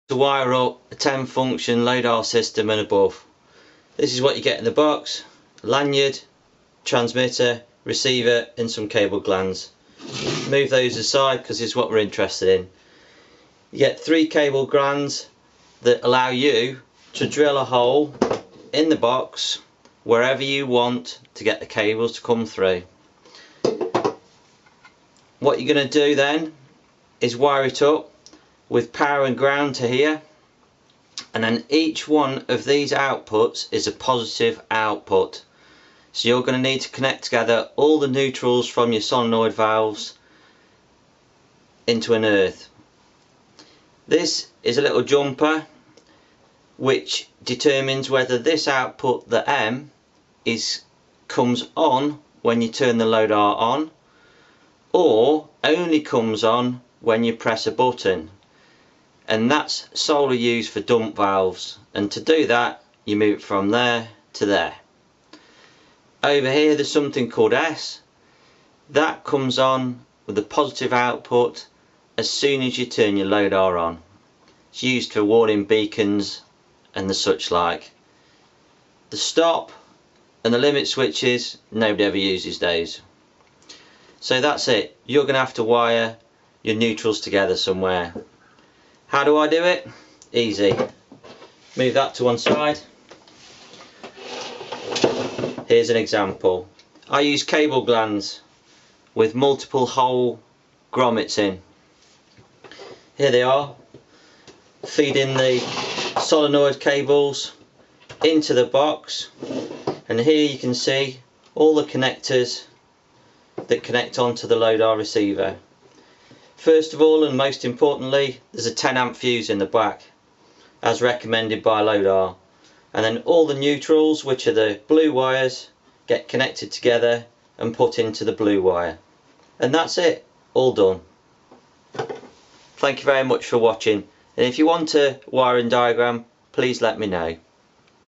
ELP with wiring a lodar remote control 10, 16 and 20 Function system
ELP have created this video to give you an overview of wiring a 10 output Lodar remote control yourself.  We created it because of the numerous questions from customers on how to deal with the neutral (ground) wires from the solenoids on their machines because the Lodar only has positive output connections